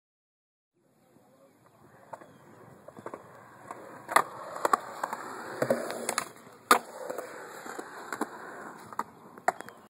percy johnsons 18o air nose grab
Percy Johnson doing a nice 180 air nose grab at berea skate park